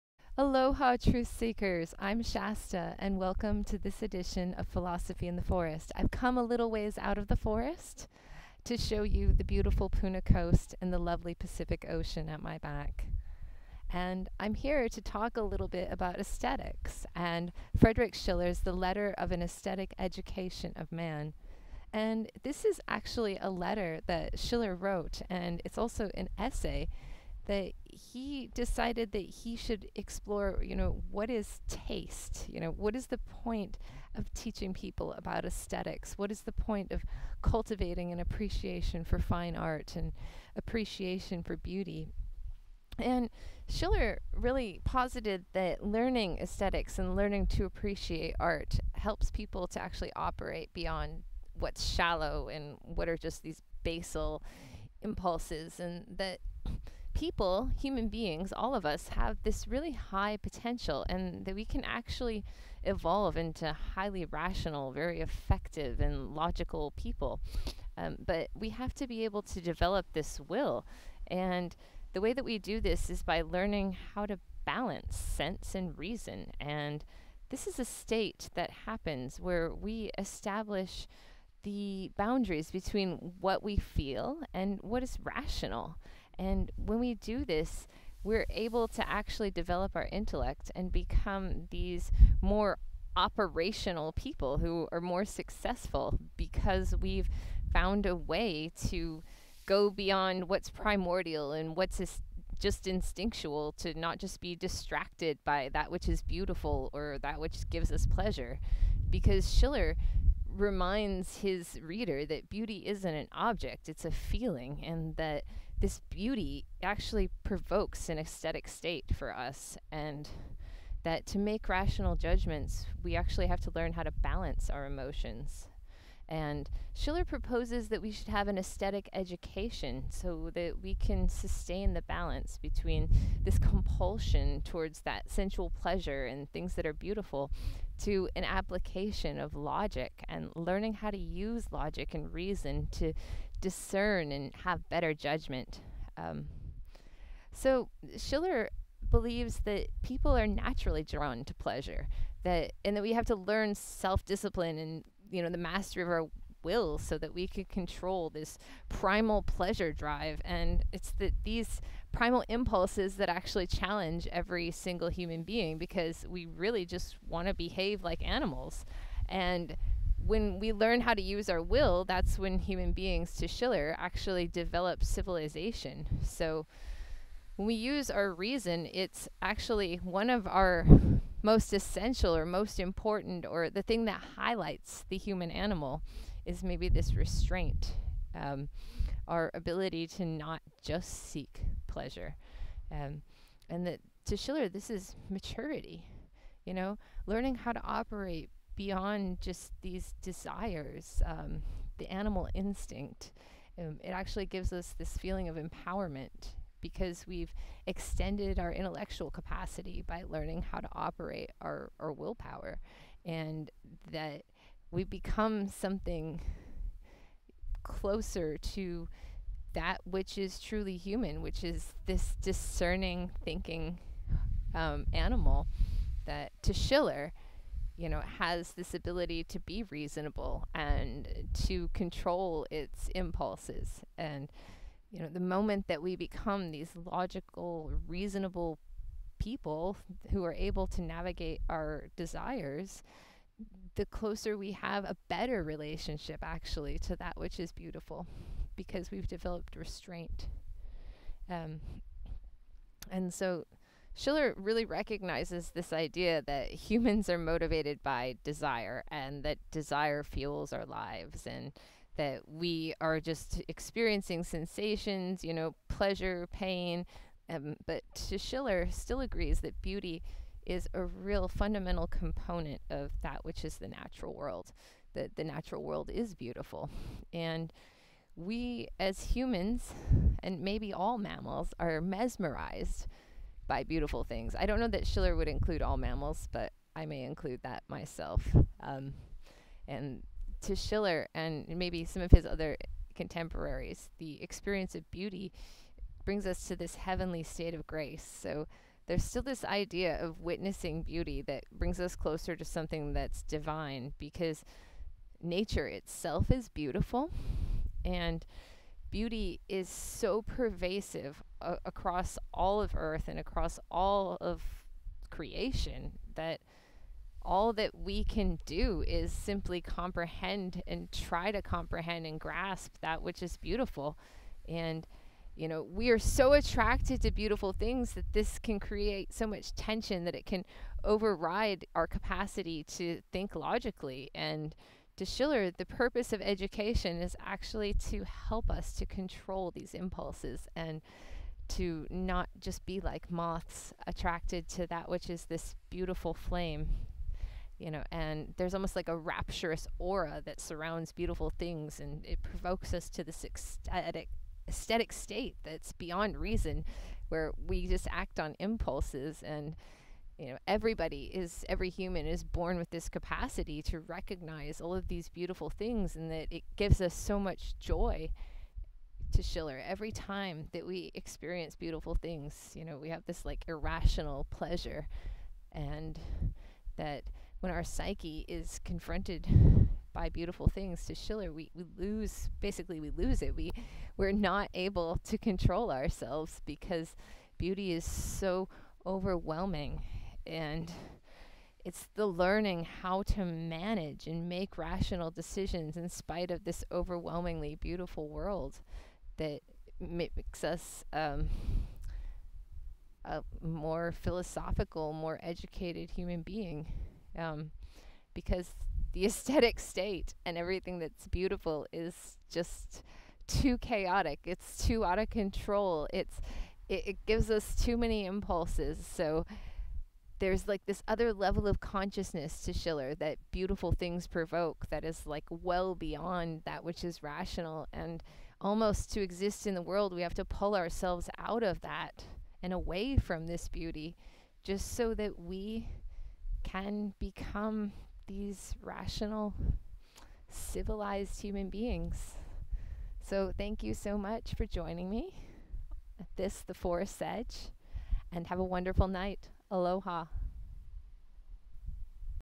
Friedrich von Schiller On the Aesthetic Education of Man (Beauty Touches the Soul)
Johann Christoph Friedrich von Schiller (1759 – 1805) was a German, poet, playwright, and philosopher. A pivotal work by Schiller was On the Aesthetic Education of Man in a Series of Letters (Über die ästhetische Erziehung des Menschen in einer Reihe von Briefen), first published 1794, which was inspired by the great disenchantment Schiller felt about the French Revolution, its degeneration into violence and the failure of successive governments to put its ideas into practice. Schiller wrote that "a great moment has found a little people"; he wrote the Letters as a philosophical inquiry into what had gone wrong, and how to prevent such tragedies in the future. In the Letters he asserts that it is possible to elevate the moral character of a people, by first touching their souls with beauty, an idea that is also found in his poem Die Künstler (The Artists): "Only through Beauty's morning-gate, dost thou penetrate the land of knowledge."